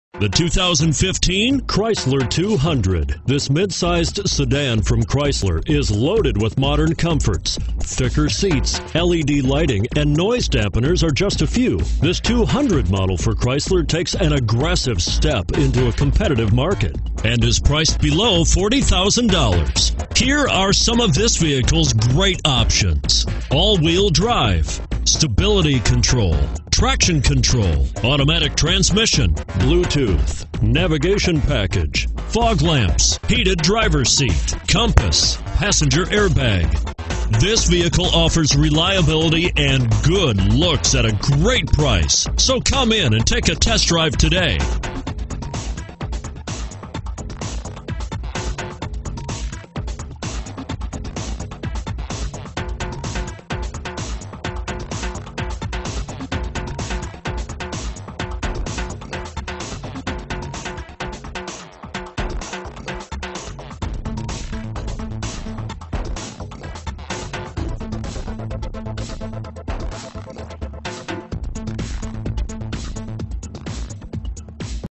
2015 Chrysler 200 Carmel NY 15725
2015 Chrysler 200 C - $35380

Safety equipment includes: ABS, Traction control, Passenger Airbag, Curtain airbags, Front fog/driving lights...Other features include: Leather seats, Bluetooth, Power locks, Power windows, Heated seats...New Inventory!!! All Wheel Drive!!! Special Financing Available: APR AS LOW AS 0% OR REBATES AS HIGH AS $4,000... Optional equipment includes: Dual Pane Panoramic Sunroof, Navigation & Sound Group I, Wheels: 19' x 8.0' Gray Polished Spoke Aluminum...